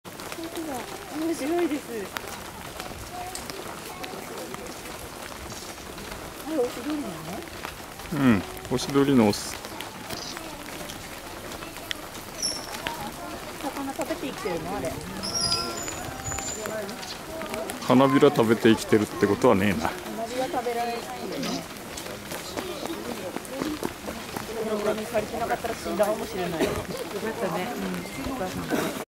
弘前公園 桜の花びらの海を泳いで行くオシドリ
桜の名所弘前公園。五月の連休前後に満開を迎えますが、花びらが落ちたお堀の表面は一面真っ白です。その花びらの海を掻き分けてオシドリが泳いで行きます。2012年5月4日撮影